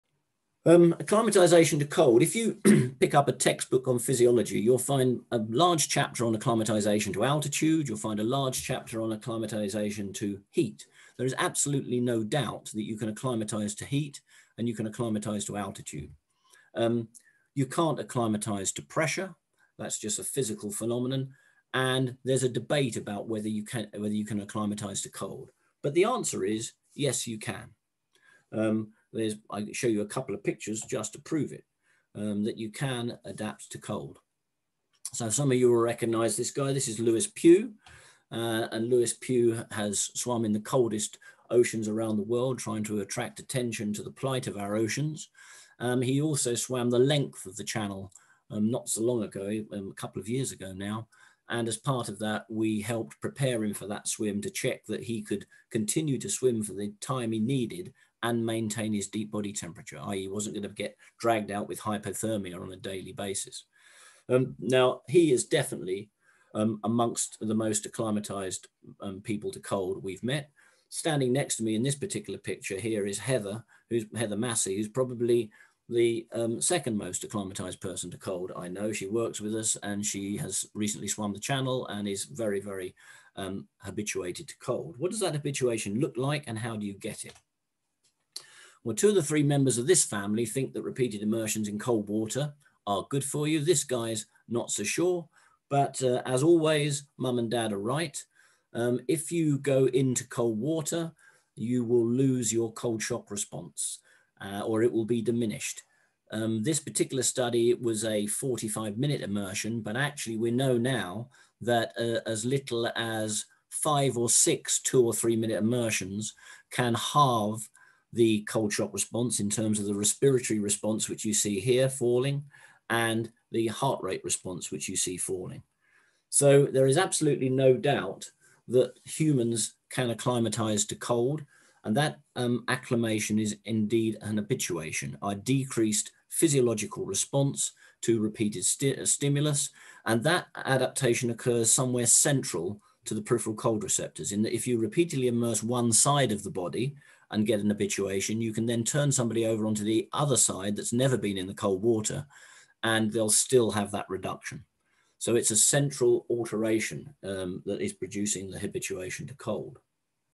Professor Mike Tipton live webinar - Acclimatisation to cold
A short clip from Mikes live webinar with Norma and myself talking about cold water immersion and habituation - can humans adapt to cold water?